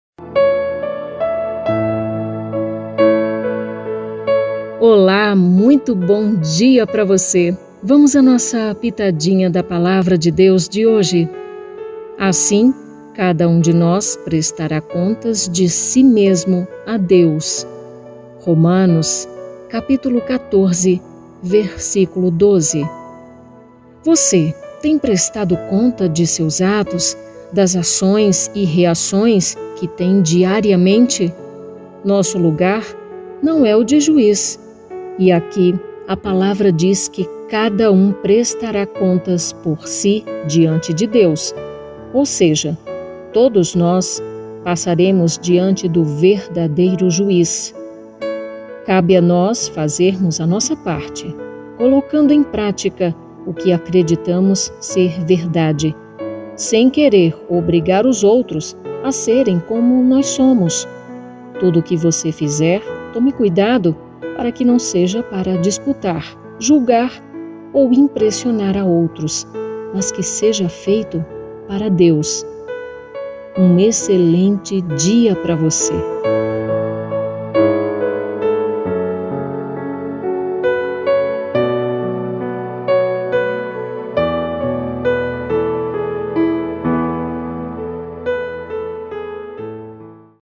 Pitadinha da Palavra de Deus 25 de setembro
“Assim, cada um de nós prestará contas de si mesmo a Deus”. (Romanos, 14:12)
Você tem prestado conta de seus atos, das ações e reações que tem diariamente?
Nosso lugar não é o de Juiz, e aqui a palavra diz que cada um prestará contas por si diante de Deus, ou seja, todos nós passaremos diante do verdadeiro juiz.
Cabe a nós fazermos a nossa parte, colocando em pratica o que acreditamos ser verdade, sem querer obrigar os outros a serem como nós somos.
Tudo o que você fizer, tome cuidado para que não seja para disputar, julgar, ou impressionar a outros, mas que seja feito para Deus!
Bom dia.